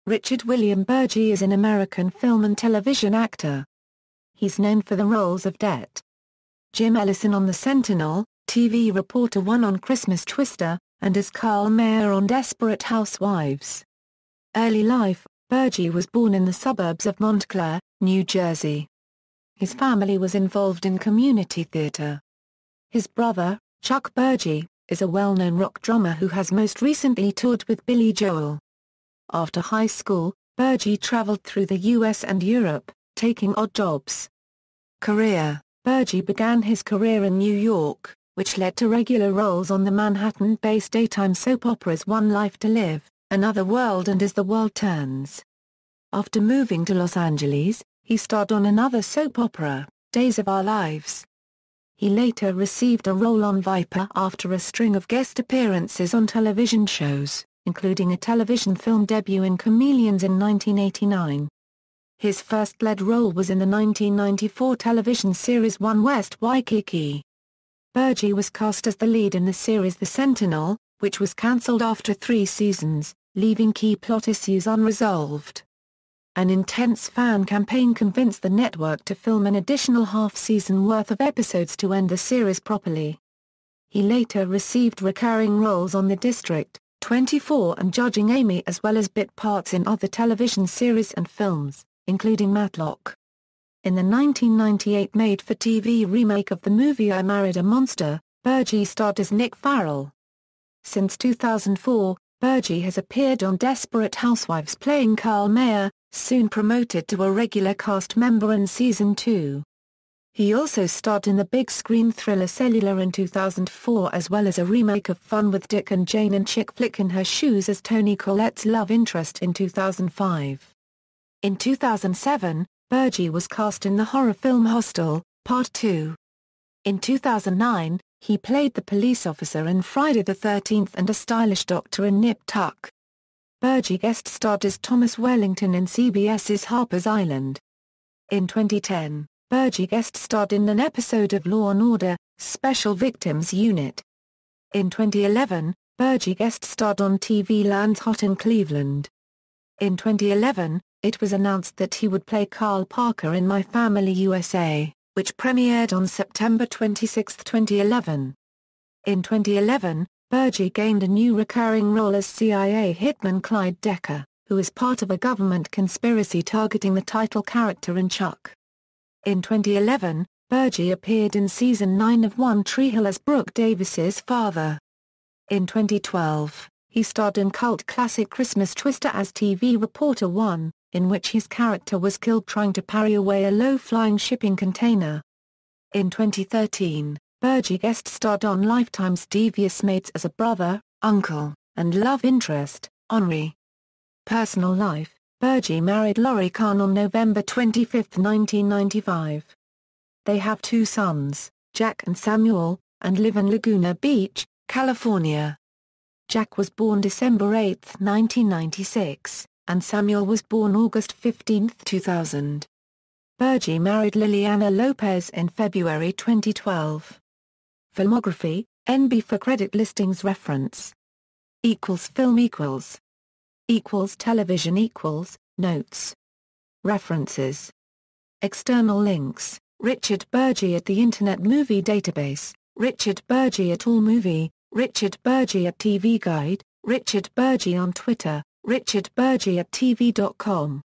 Richard Burgi
Richard William Burgi is an American film and television actor. He's known for the roles of Det. Jim Ellison on The Sentinel, TV Reporter 1 on Christmas Twister, and as Karl Mayer on Desperate Housewives.